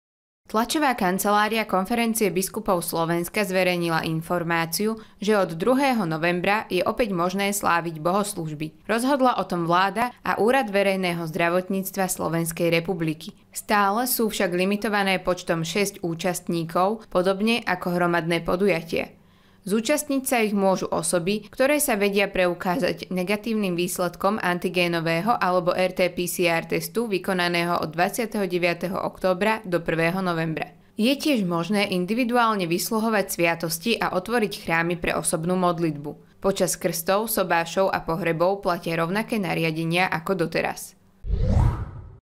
KOSTOLY SÚ OPÄŤ OTVORENÉ
Veriaci v našom regióne sa opäť budú môcť osobne zúčastniť bohoslužieb. Podmienkou je negatívny výsledok testu na COVID-19.